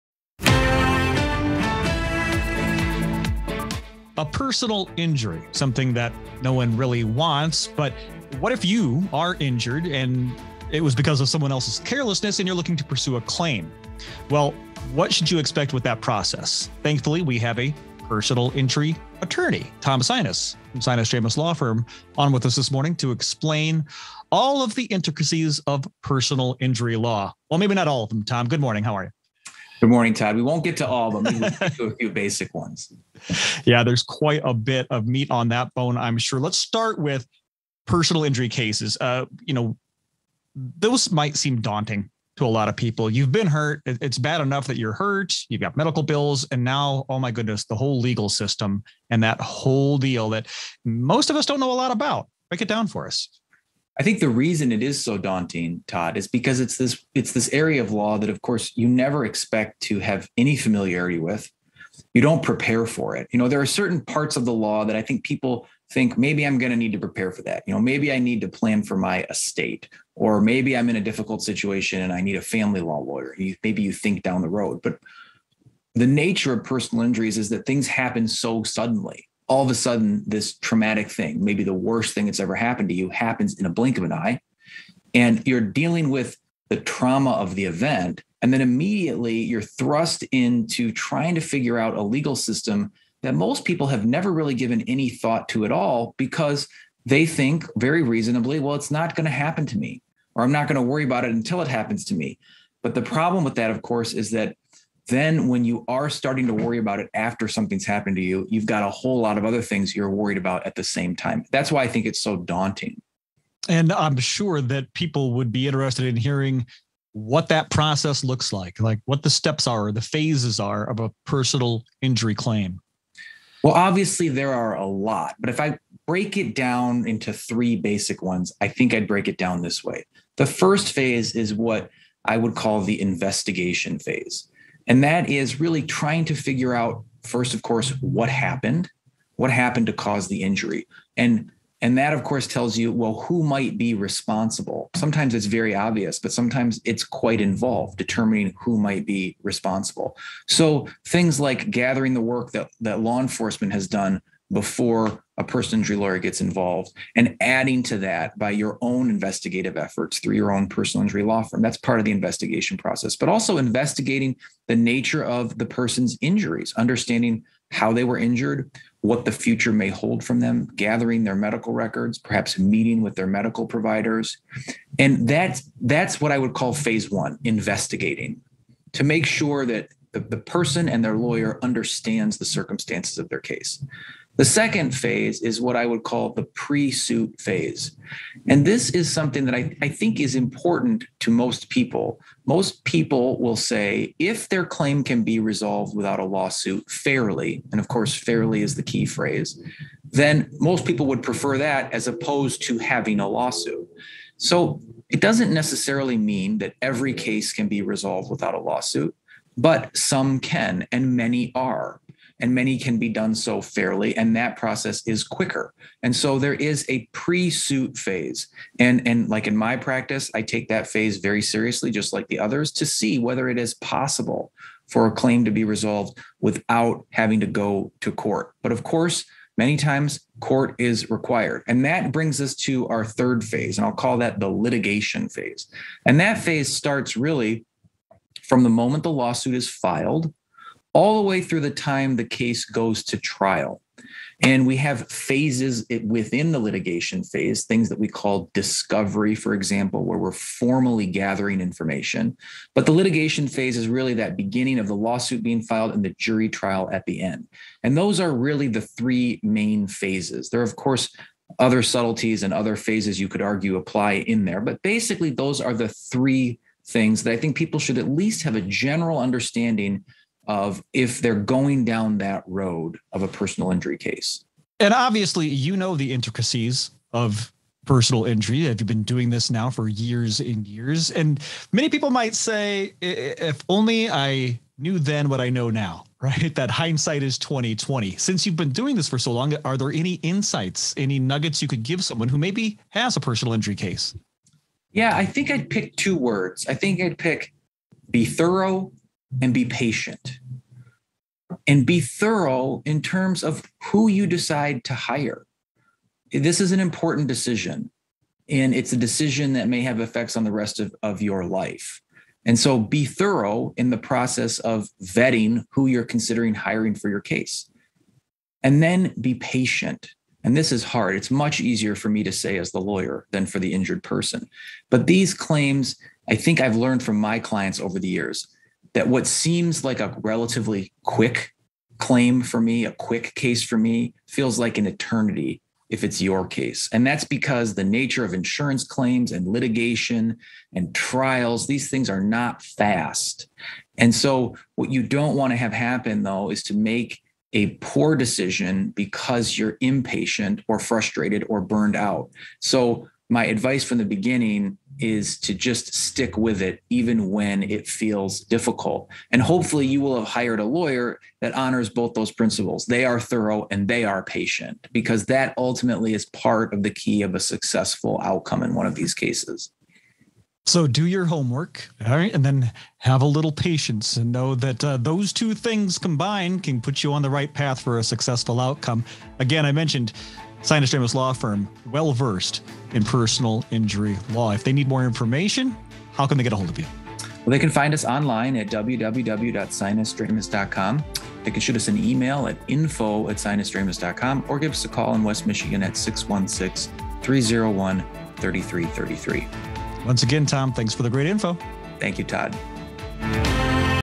Know the Law: Personal Injury Claim Basics
A personal injury claim is something no one wants to have. But what if you are injured because of someone else’s carelessness and you want to pursue a claim? What should you expect about the process? This week, Grand Rapids personal injury lawyer, Tom Sinas, provides an overview of this type of legal claim, including investigation, pre-suit settlement efforts, and litigation.